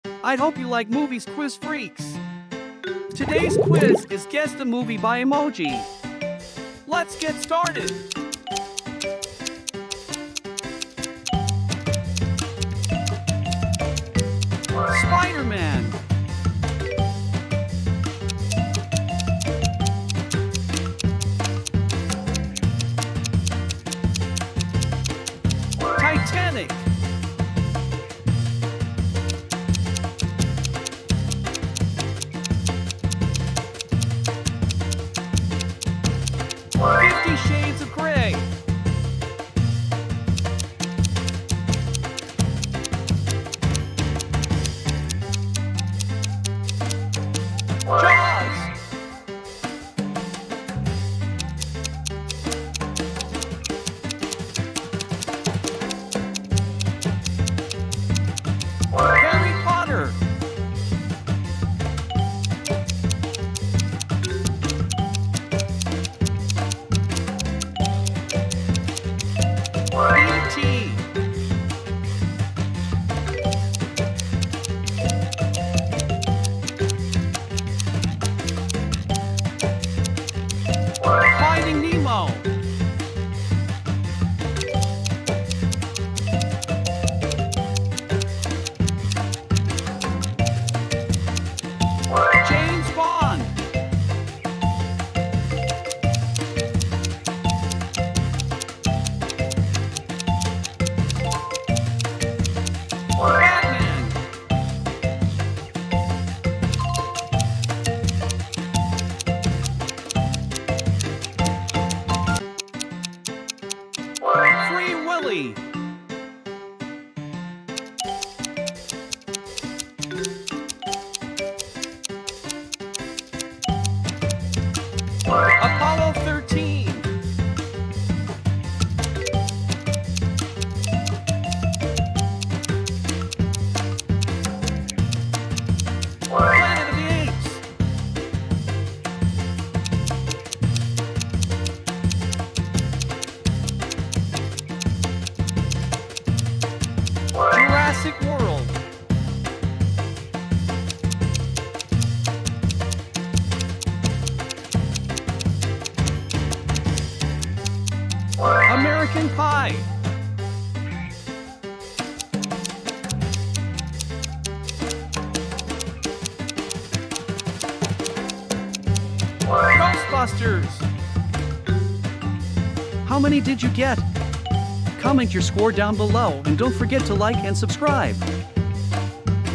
Guess the movie by EMOJI QUIZ
Do you like movies? In today's QUIZ is 15 movies described only with EMOJIS. Can you guess them all?